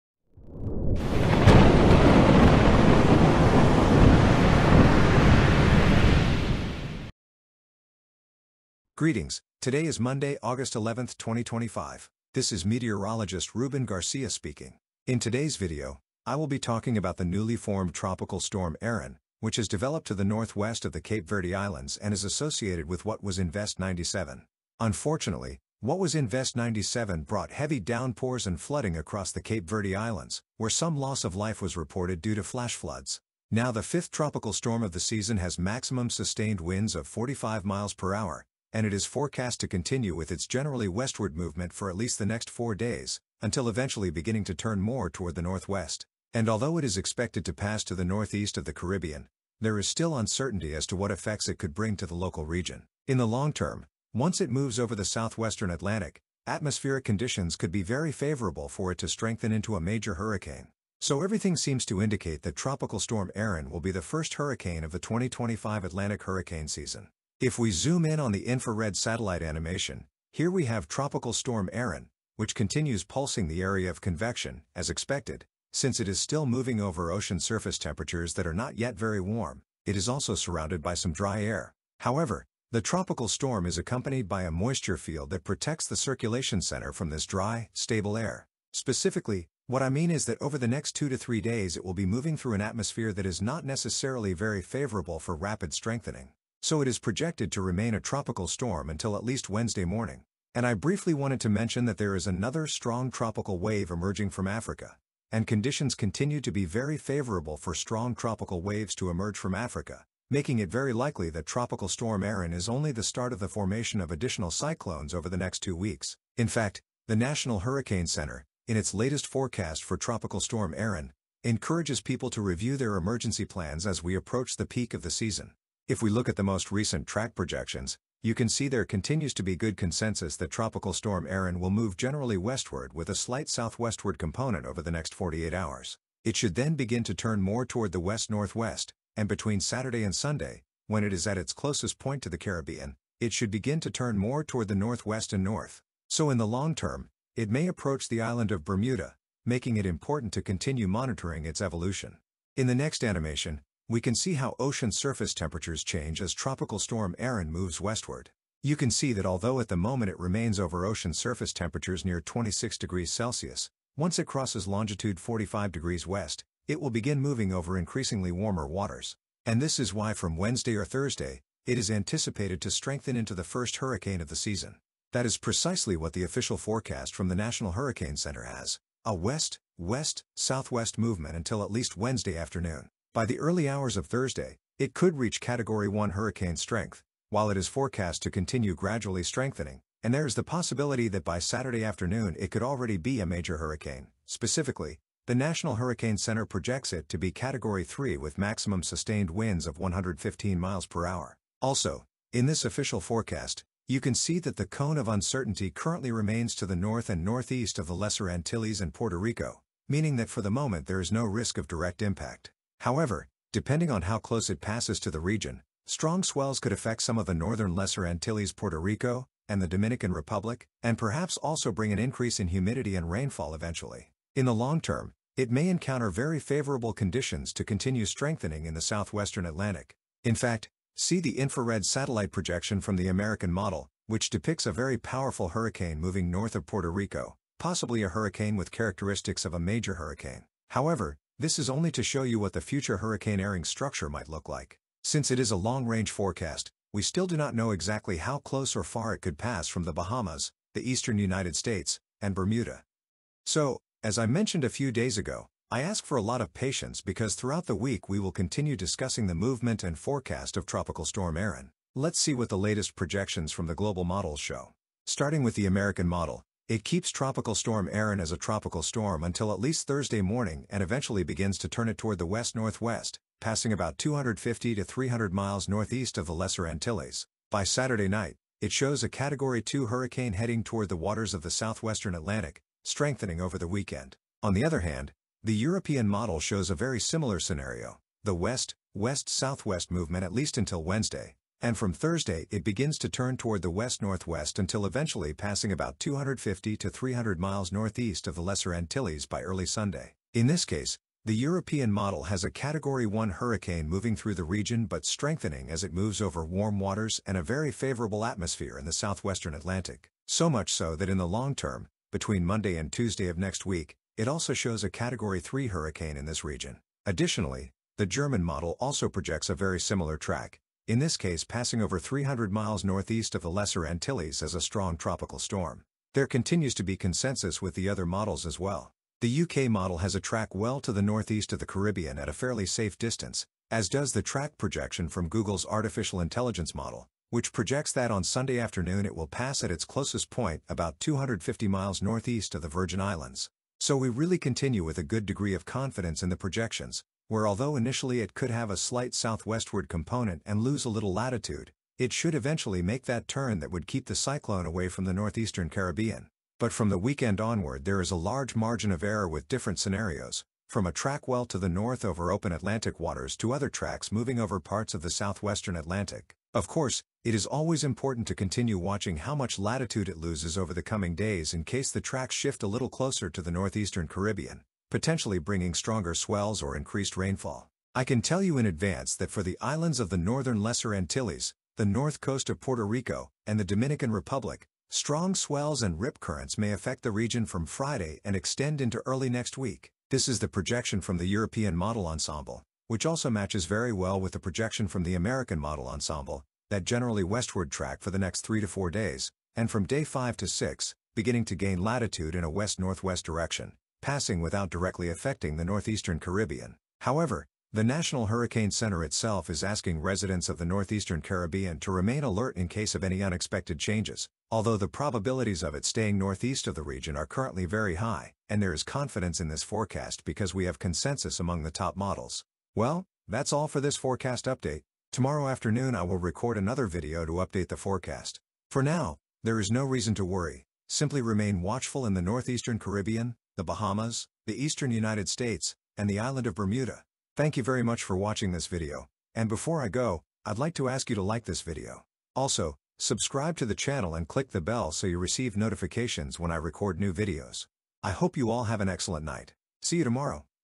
Erin could strengthen into a Category 3 hurricane. Tropical Storm Erin.
Invest 97L has strengthened into Tropical Storm Erin. It is forecast to maintain a mostly westward track until passing northeast of the Caribbean over the weekend.

Conditions are favorable for it to gradually strengthen over the next few days, reaching Category 3 hurricane status when it passes at its closest point to the northeast Caribbean.